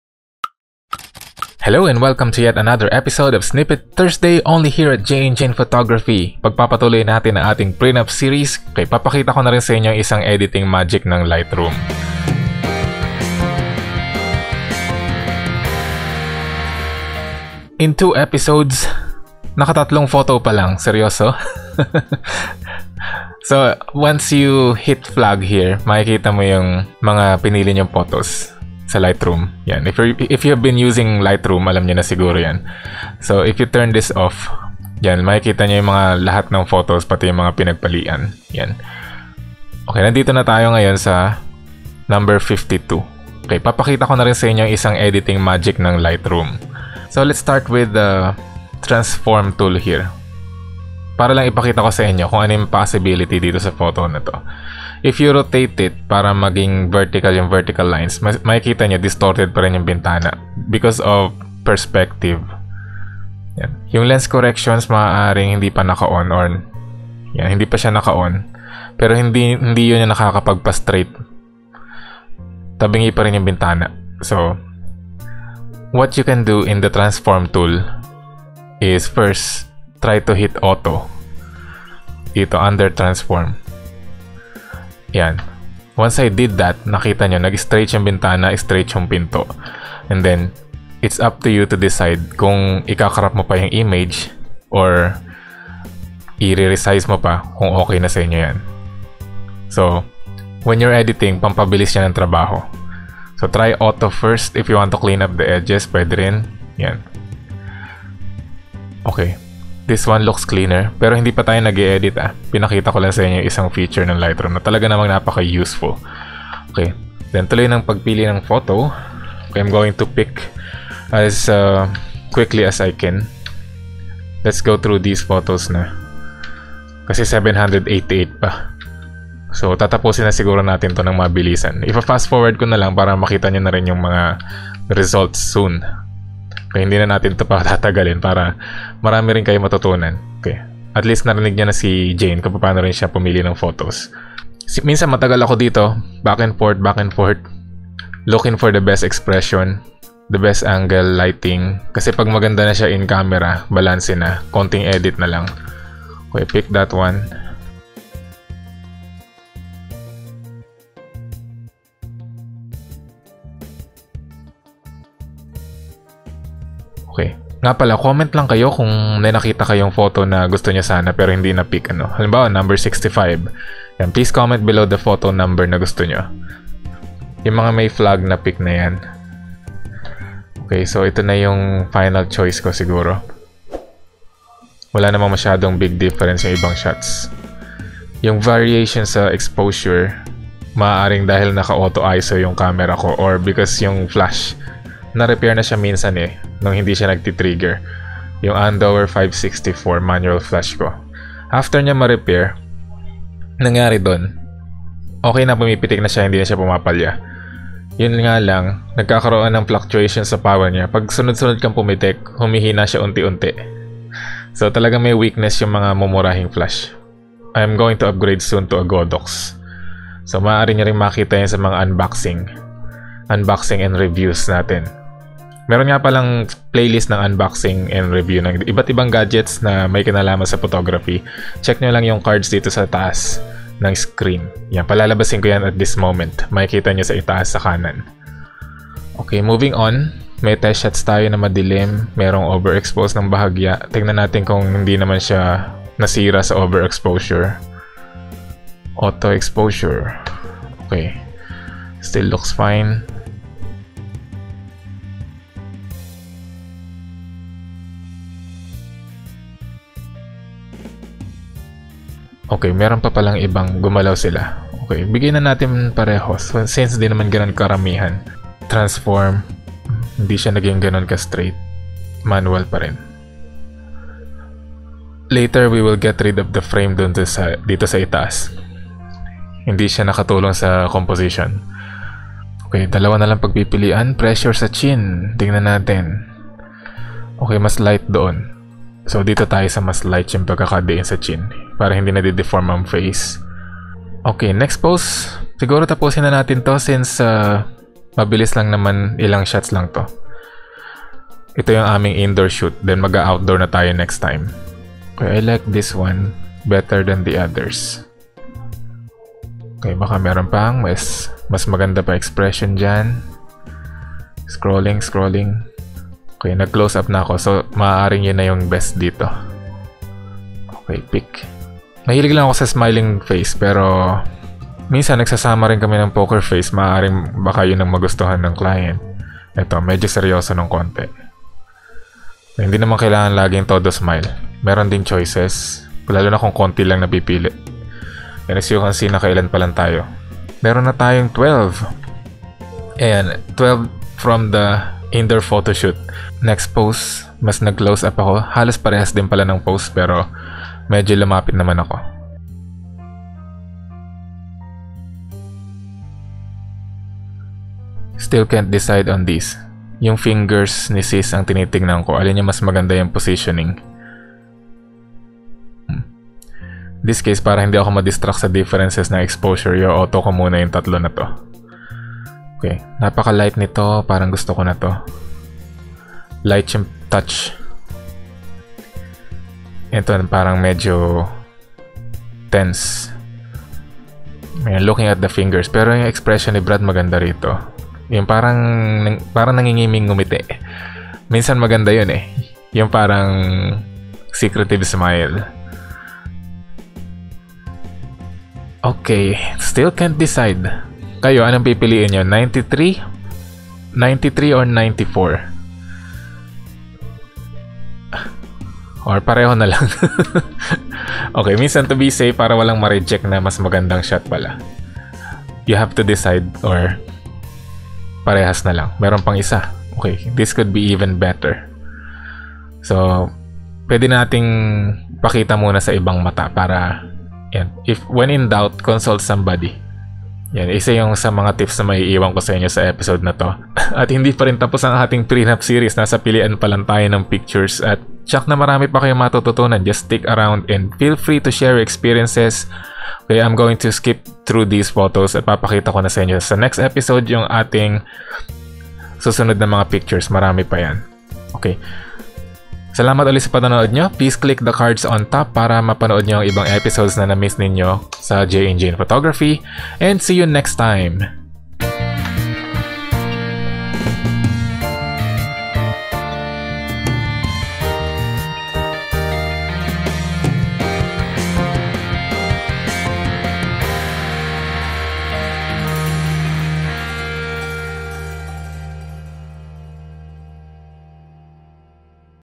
Lightroom Classic | Basics of Selecting Photos | Tagalog Tips | Jay&Jane Photography
MORE tips sa pagpili ng BEST photos sa Lightroom! Ituloy natin ang prenup series! Keep watching and learn more!

Snippet Thursday #26 (04/05/2021)